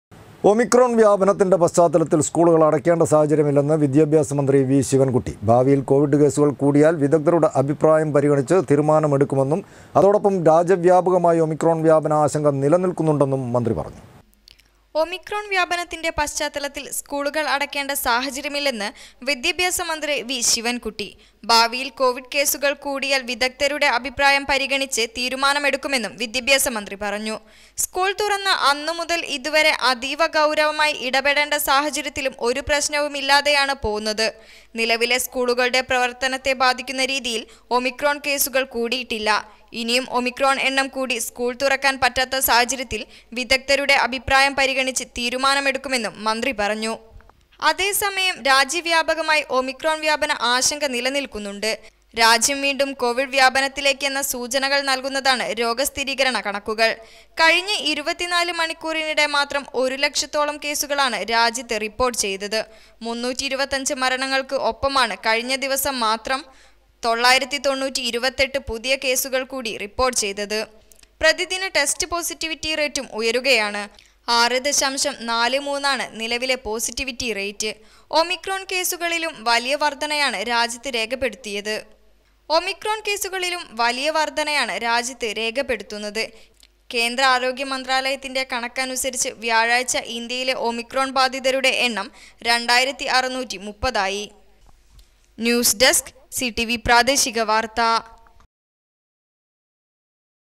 ഒമിക്രോണ്‍ വ്യാപനത്തിന്റെ പശ്ചാത്തലത്തില്‍ സ്‌കൂളുകള്‍ അടയ്‌ക്കേണ്ട സാഹചര്യമില്ലെന്ന്
ഒമിക്രോണ്‍ വ്യാപനത്തിന്റെ പശ്ചാത്തലത്തില്‍ സ്‌കൂളുകള്‍ അടയ്‌ക്കേണ്ട സാഹചര്യമില്ലെന്ന് വിദ്യാഭ്യാസ മന്ത്രി വി ശിവന്‍കുട്ടി. ഭാവിയില്‍ കൊവിഡ് കേസുകള്‍ കൂടിയായില്‍ വിദഗ്ധരുടെ അഭിപ്രായം പരിഗണിച്ച്‌ തീരുമാനമെടുക്കുമെന്നും, അതോടൊപ്പം
 രാജ്യവ്യാപകമായി ഒമിക്രോണ്‍ വ്യാപന ആശങ്ക നിലനില്‍ക്കുന്നുണ്ട് എന്നും മന്ത്രി പറഞ്ഞു.